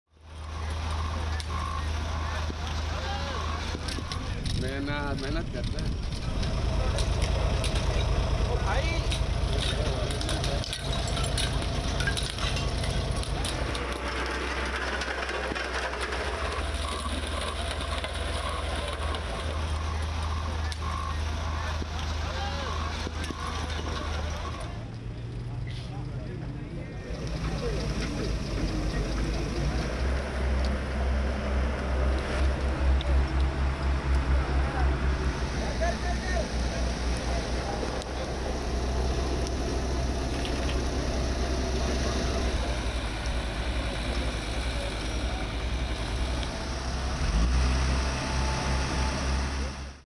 Contrebande : le Pakistan détruit 250 millions de dollars de produits
🇵🇰 NoComment | Au Pakistan, les autorités détruisent l'équivalent de 250 millions de dollars de produits de contrebande.…